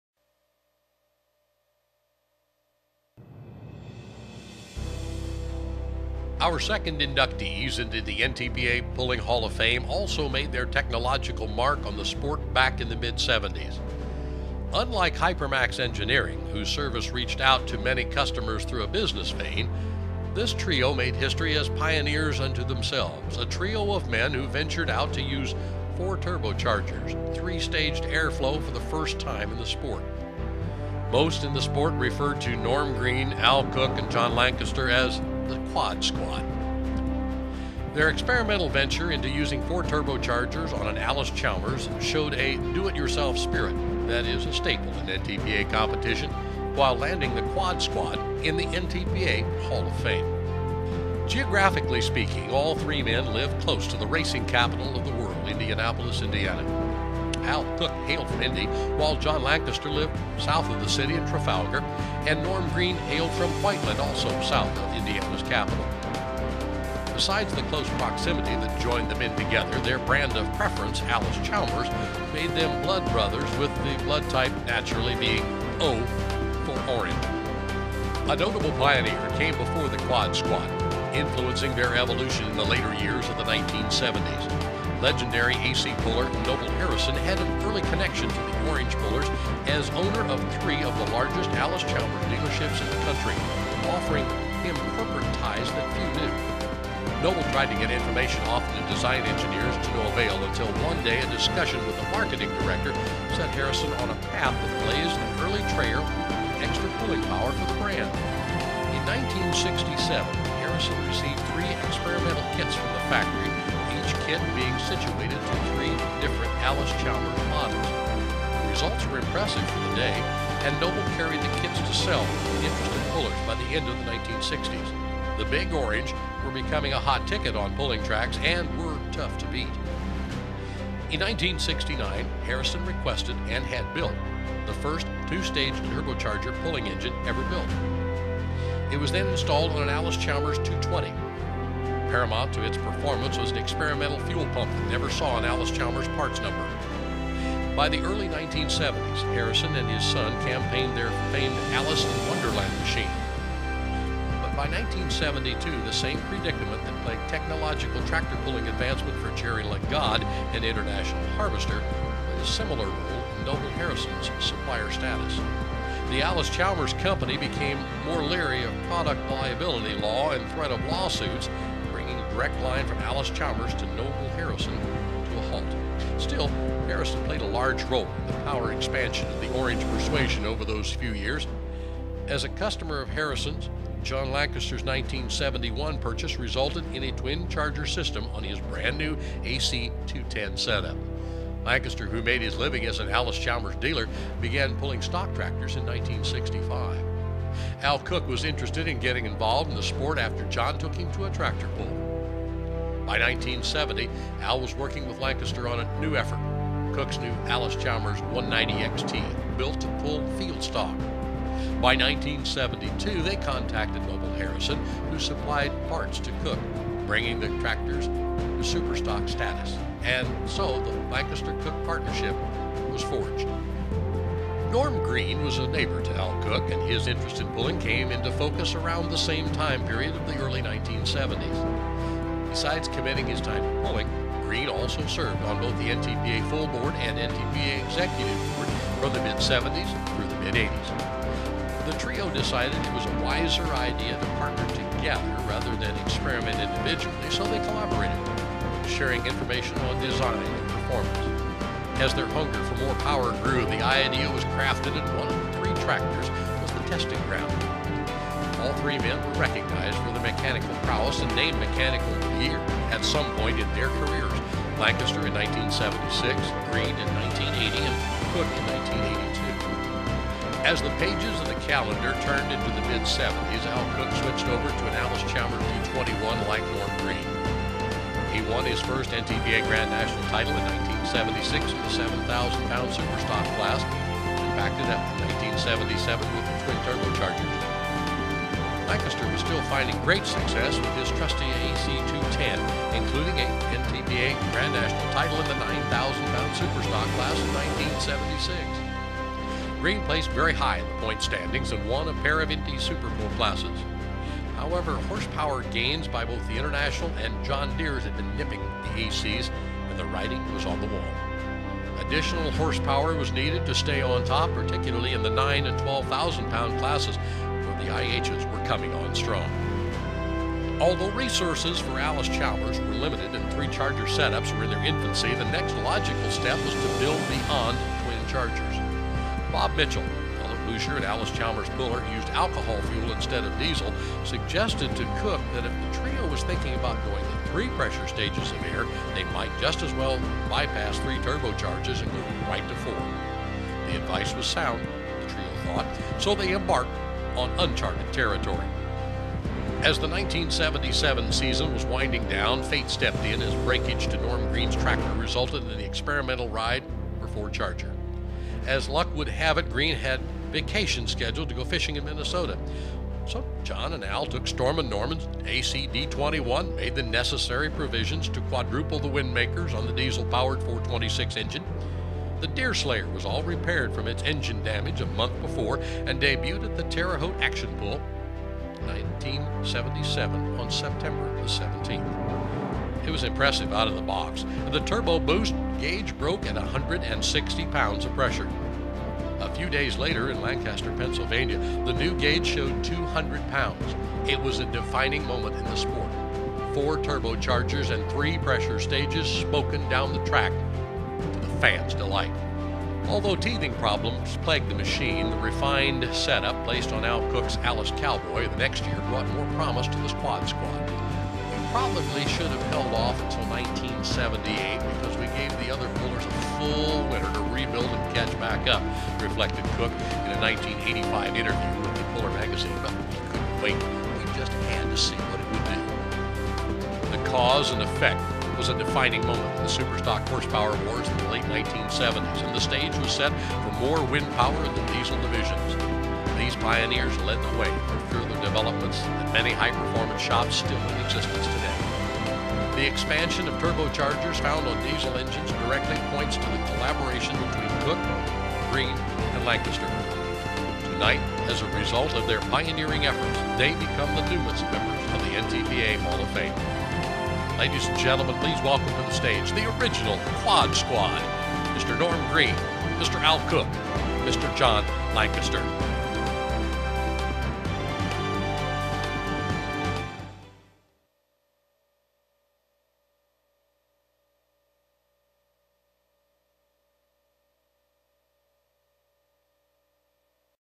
Quad Squad Intro Video 2013 NTPA awards banquet
Introduction of John Lancaster, Al Koch, and Norm Green. New inductees to the National Tractor Pullers Association (NTPA) Hall Of Fame. NTPA awards banquet 2013.